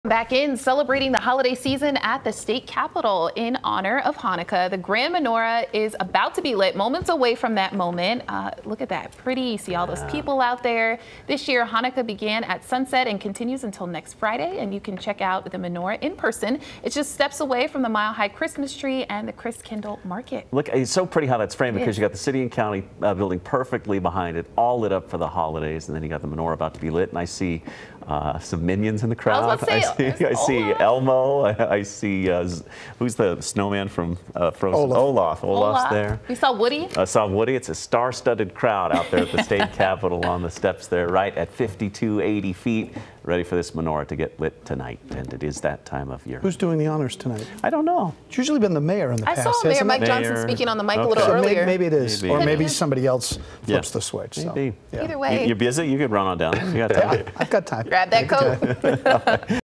Menorah lighting at Colorado Capitol
The Colorado Capitol is holding its menorah lighting for the first night of Hanukkah.